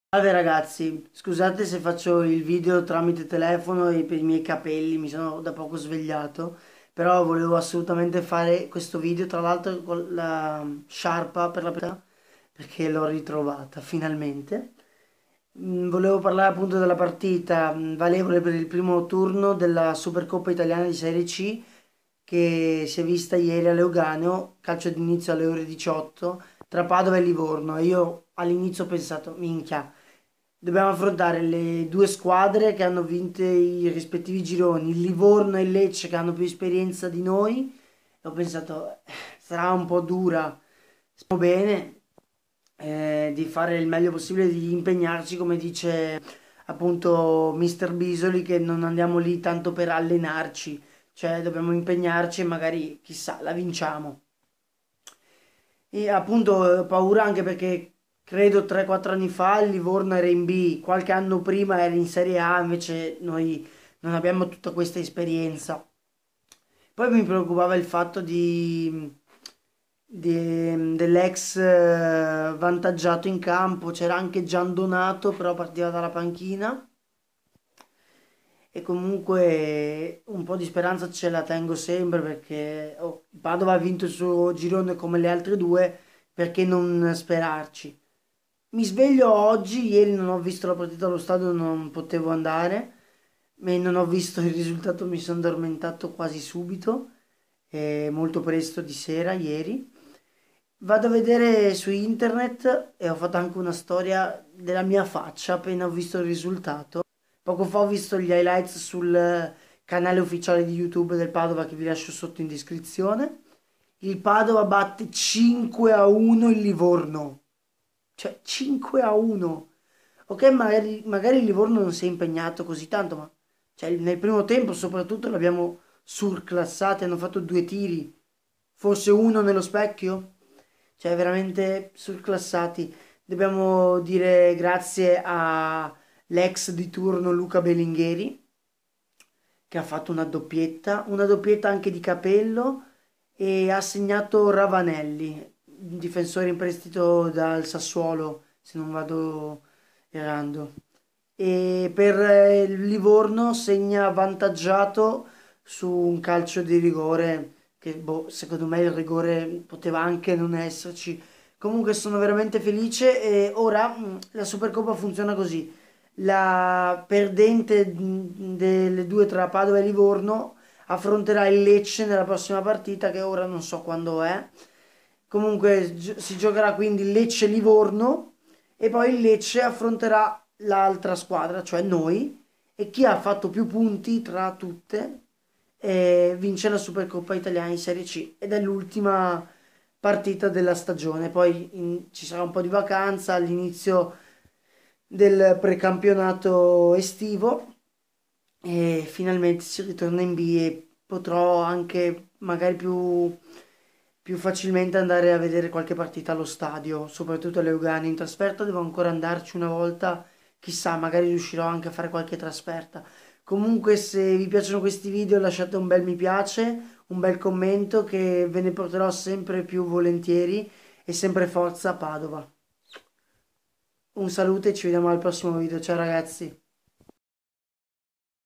ASSURDO!!! PADOVA-LIVORNO 5-1 SUPERCOPPA ITALIA DI SERIE C
Prima giornata per assegnare la Supercoppa Italia di serie C,  il Padova surclassa il Livorno  (doppiette di Belingheri e Capello, gol di Ravanelli, per il Livorno gol dal dischetto dell'ex Vantaggiato)